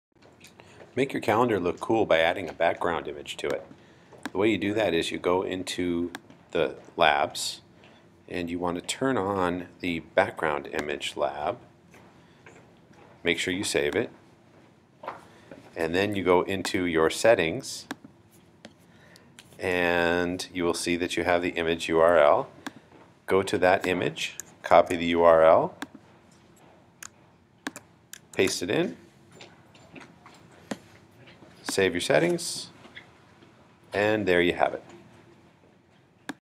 How to Add a Background Image to Google Calendar
Update: Google has discontinued labs, therefore you can no longer add a background image to your Google calendar.